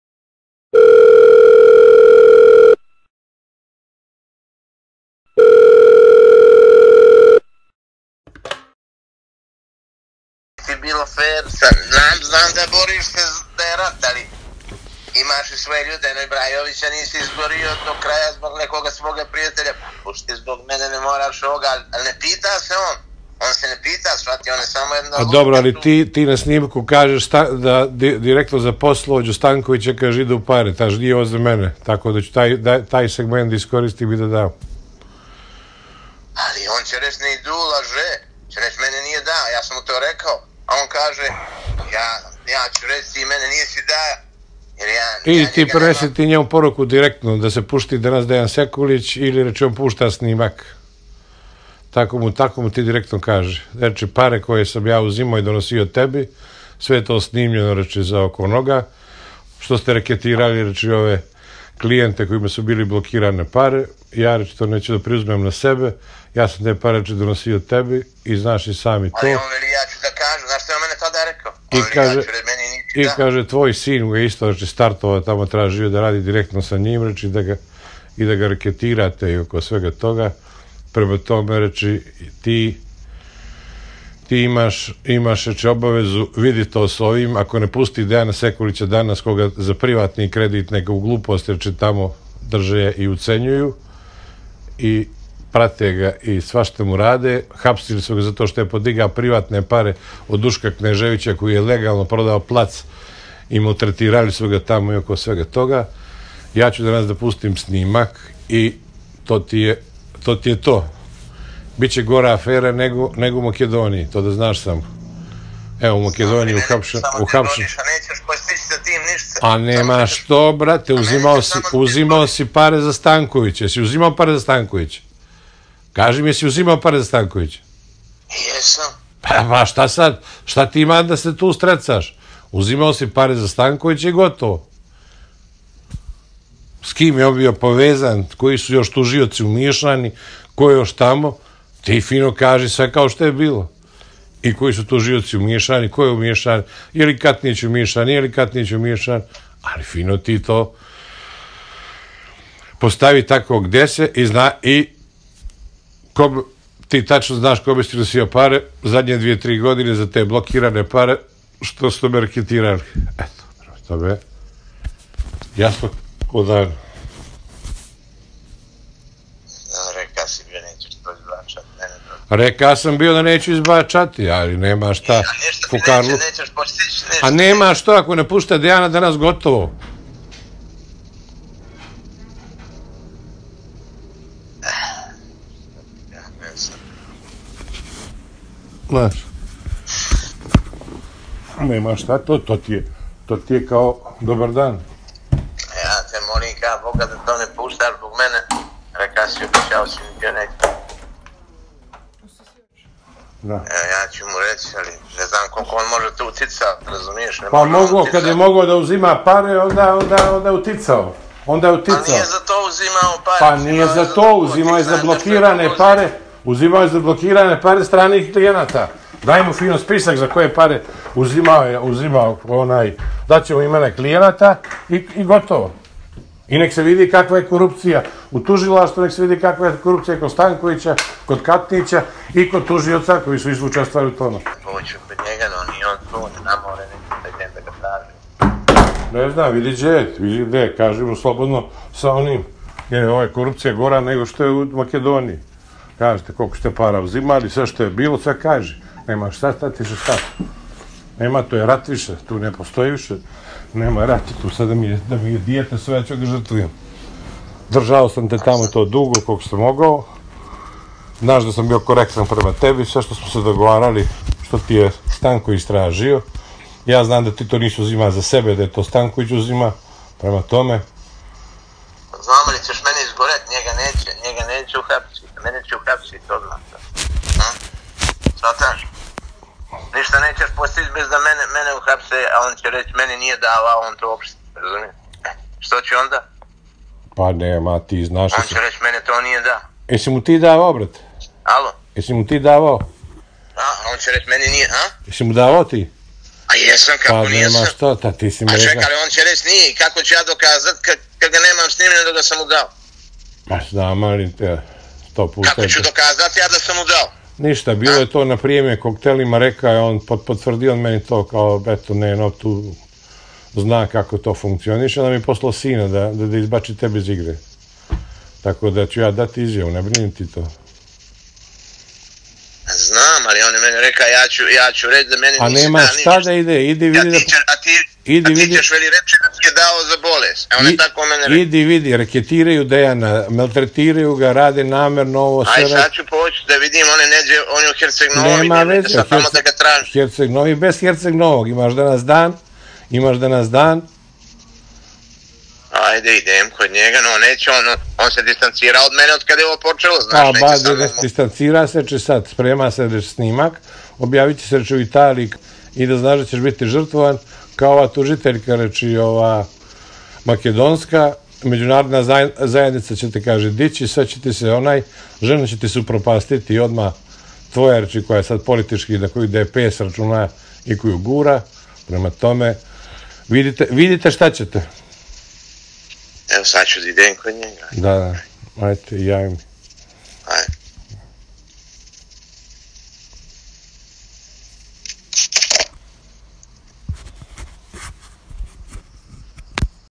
ИН4С: Вујошевић Кнежевићу: Јесам, прослеђивао сам твој новац Станковићу
Први у низу снимака, који разоткривају како се одвија корупција у црногорском тужилаштву објавио је вечерас предсједник Атлас Групе др Душко Кнежевић на свом Ју тјуб налогу.

Кнежевић у телефонском разговору разговара са секретатом Врховног државног тужилаштва Ненадом Вујошевићем Неном.